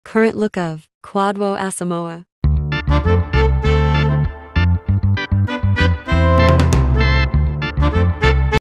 Current Look Of Former Black Star Player, Kwadwo Asamoah
In this video, we take a closer look at the current appearance of the former Black Star player, Kwadwo Asamoah. Known for his incredible skills on the field, Asamoah has left an indelible mark on the world of football.

Join us as we delve into Asamoah's transformation since his days as a professional player, exploring his current style, fashion choices, and overall demeanor. From his iconic dreadlocks to his distinctive fashion sense, we'll uncover how Asamoah's personal style has evolved over the years.

Not only will we discuss his physical appearance, but we'll also delve into Asamoah's post-football career, shedding light on his current ventures and activities. From his philanthropic endeavors to his involvement in the sports industry, we'll explore how Asamoah continues to make an impact even after retiring from professional football.

Whether you're a die-hard fan of Asamoah or simply curious about his current look and lifestyle, this video is a must-watch. Join us on this exciting journey as we uncover the current look of the former Black Star player, Kwadwo Asamoah, and discover what he's been up to since hanging up his boots. Don't miss out on this exclusive glimpse into the life of one of football's most beloved figures!